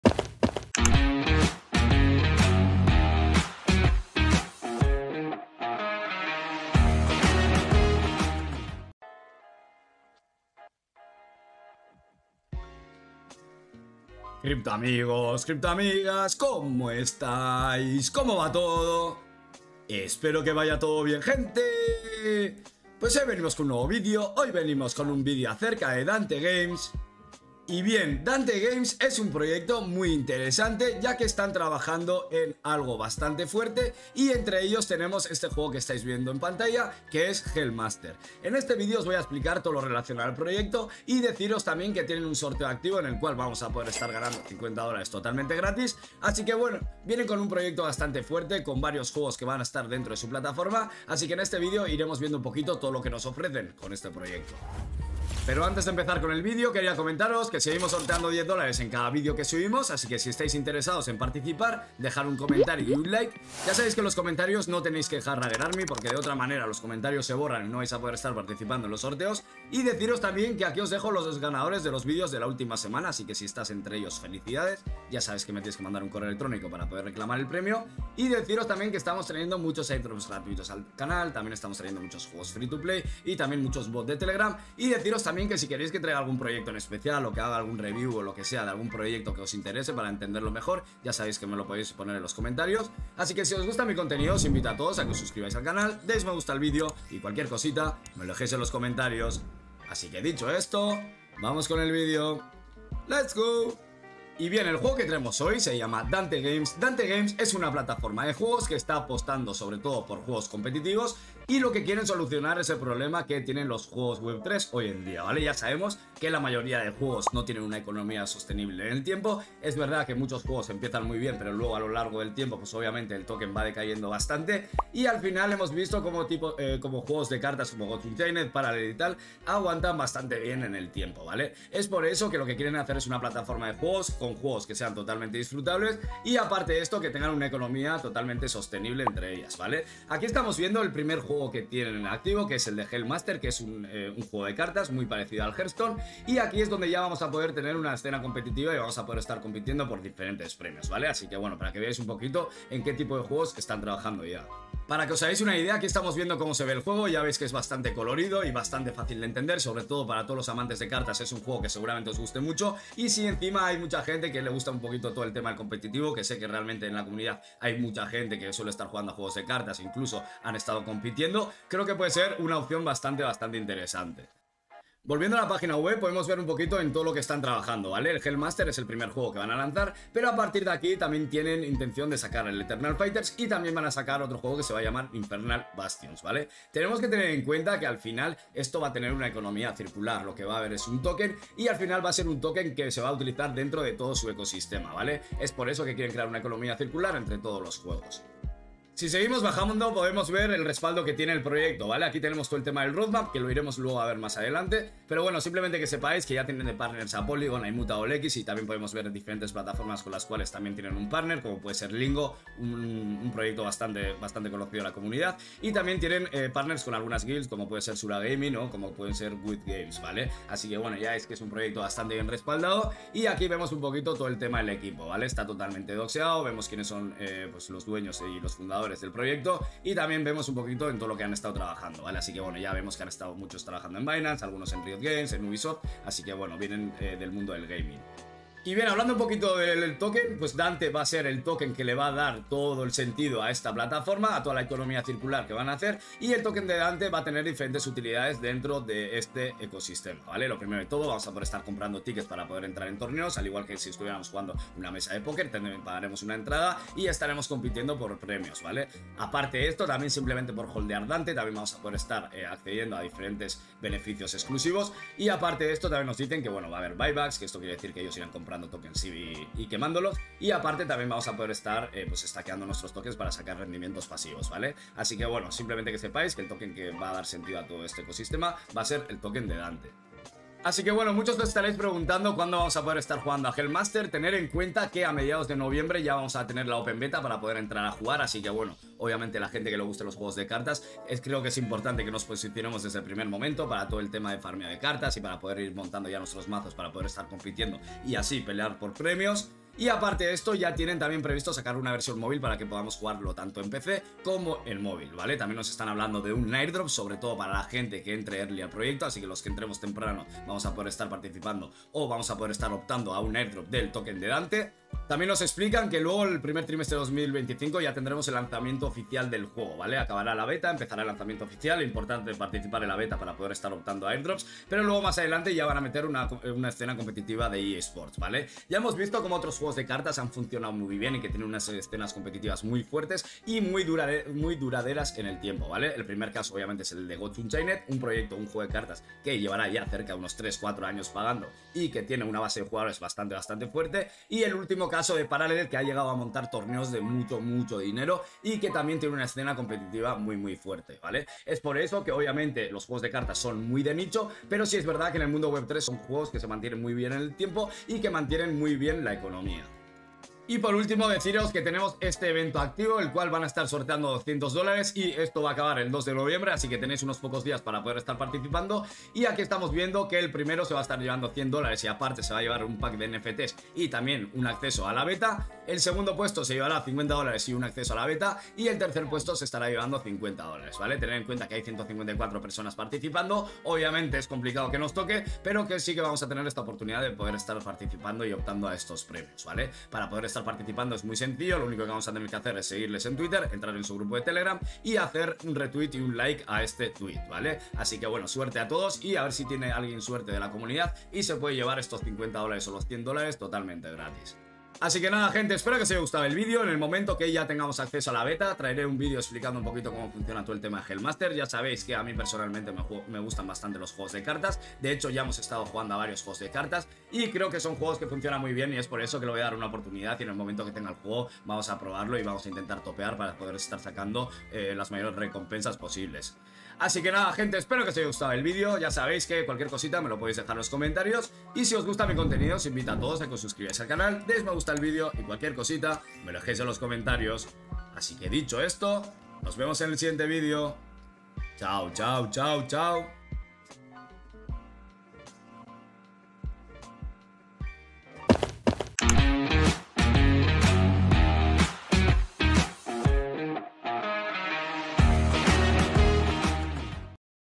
🚀 PUEDES GANAR HASTA 50$ GRATIS CON ESTE NUEVO JUEGO NFT 💥 PLATAFORMA DE JUEGOS 🚀 DANTE GAMES
En este video explico la plataforma DANTE GAMES y su primer juego NFT competitivo Hellmaster. Tambien os explico el sorteo de 200USDT que tienen activo

Recordar qué nada de lo que se dice en este video es consejo de inversión. Cada uno tiene que ser responsable de sus decisiones a la hora de invertir.